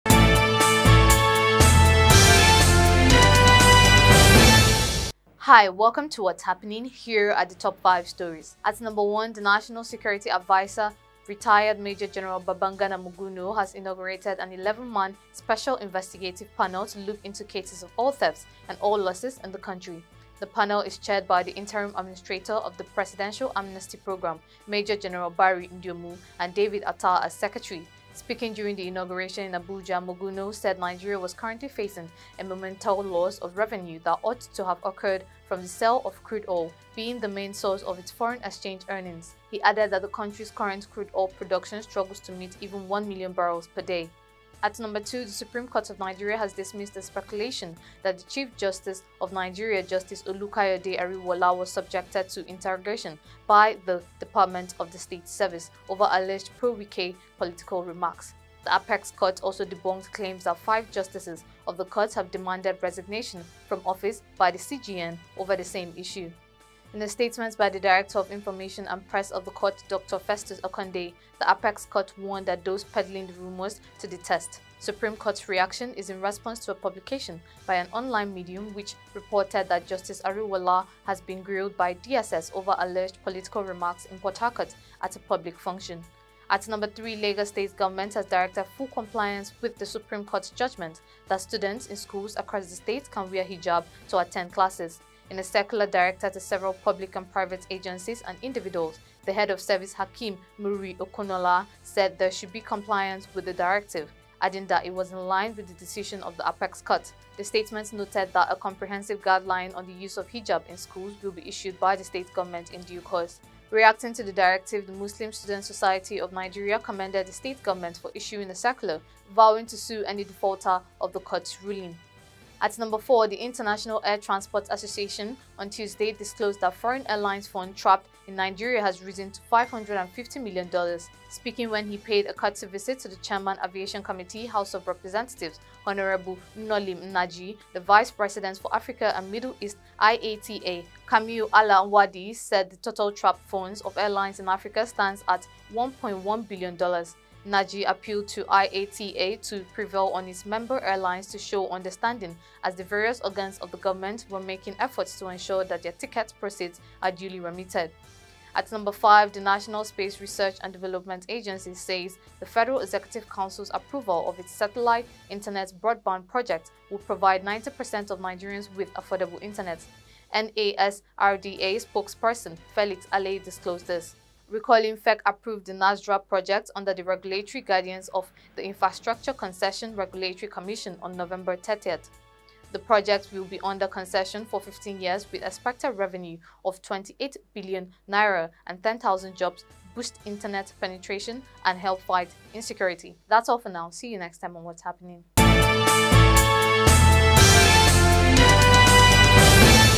FG sets up panel to probe oil theft
The National Security Adviser, Major General Babagana Monguno (retd.), has inaugurated an 11-man Special Investigative Panel to look into cases of oil theft and oil losses in the country.